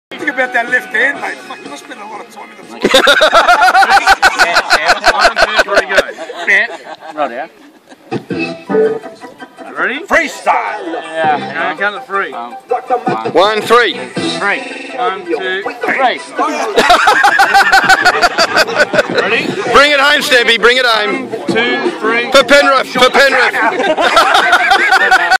Stempy versus Tex
Stempy and Tex have an arm wrestle at Valla.